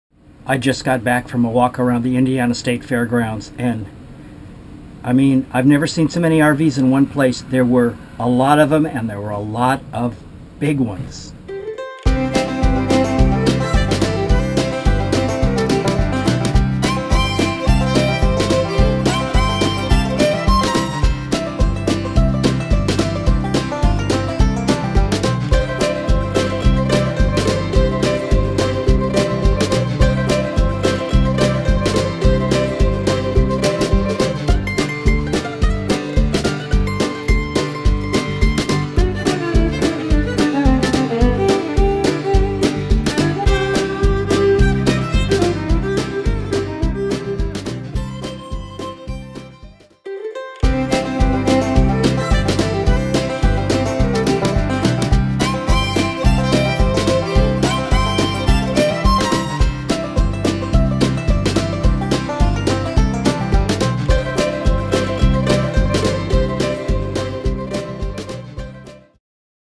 Family Motor Coach Association 2012 Rally
RVtravel.com editor Chuck Woodbury took a quick walk around the Indiana State Fairgrounds on Day Two of the FMCA International Rally. There were a lot of big RVs, and what he saw was only a fraction of the total number of them at the giant rally.